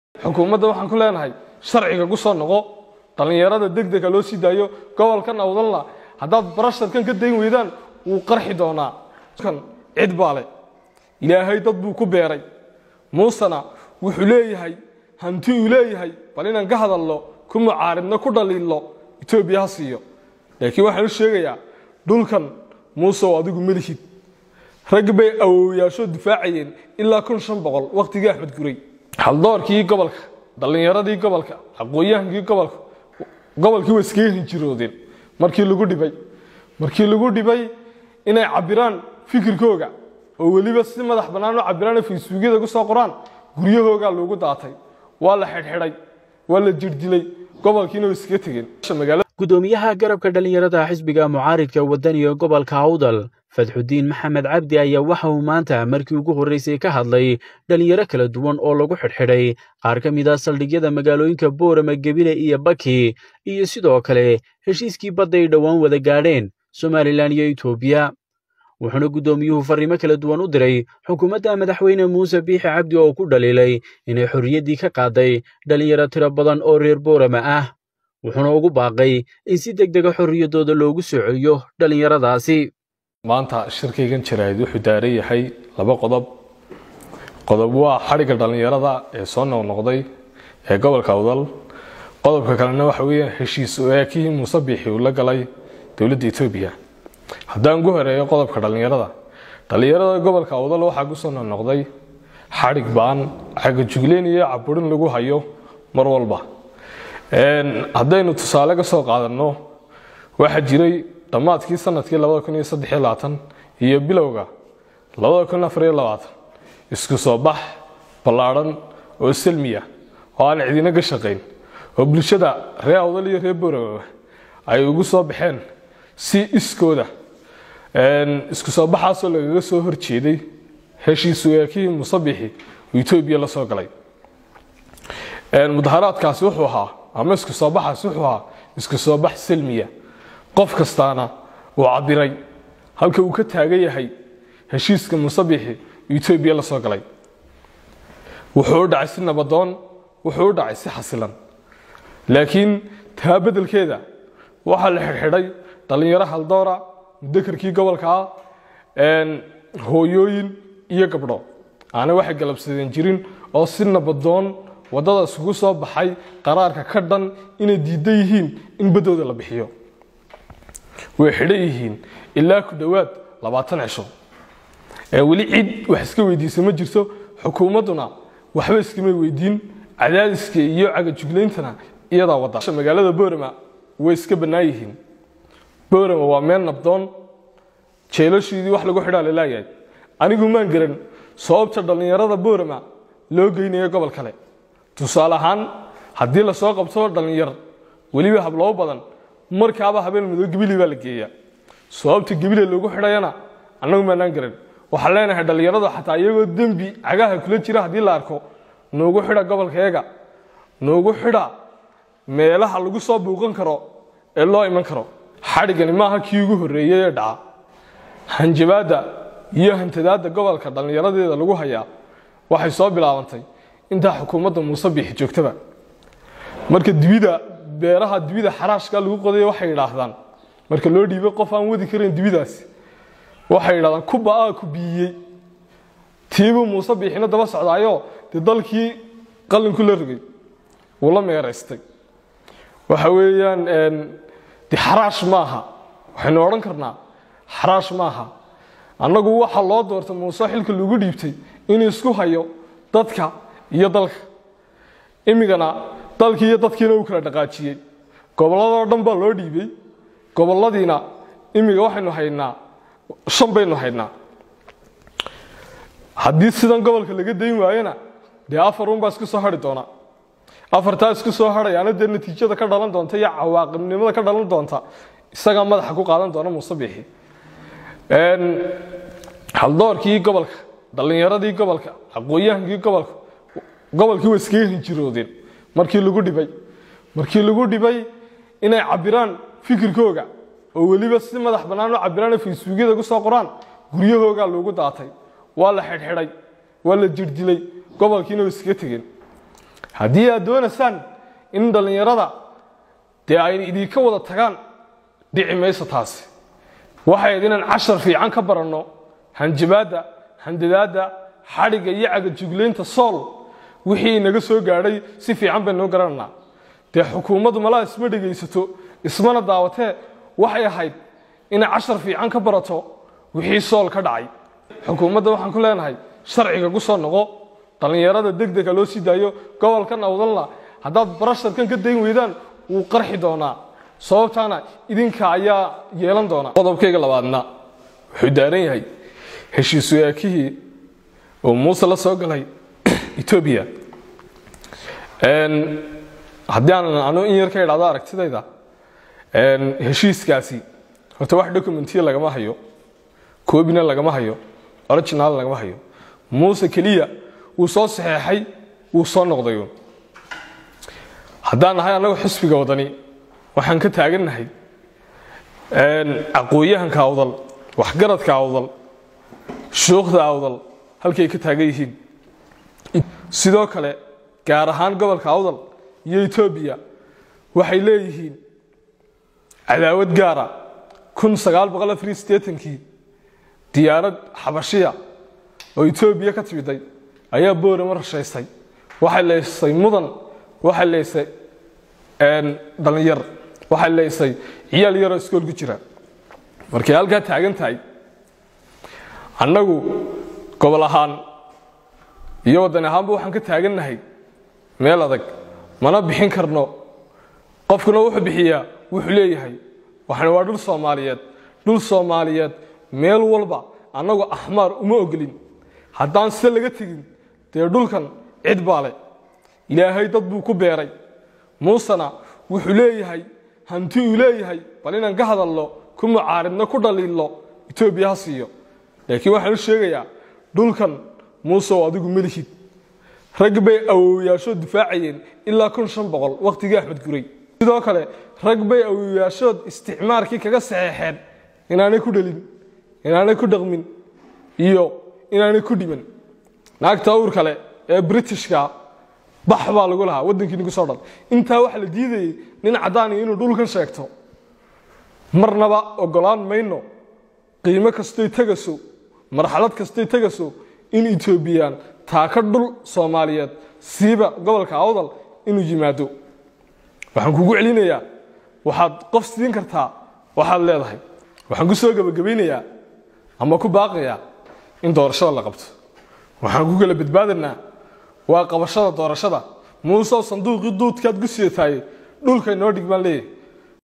Muusoow Dhulkan Adigu Ma Lihid, Waa Dhul Soomaaliyeed - Somali Show
Muusoow Dhulkan Adigu Ma Lihid, Waa Dhul Soomaaliyeed Dhalinyartana Xoriyada U Soo Celi !! Gudoomiye Fadxi diin.